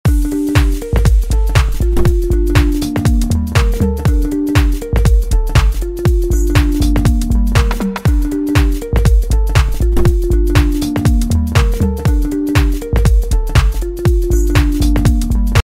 G art S painting #021 | Power lines at sunset painting | Acrylic painting
The process of painting the sunset with acrylic paints on canvas.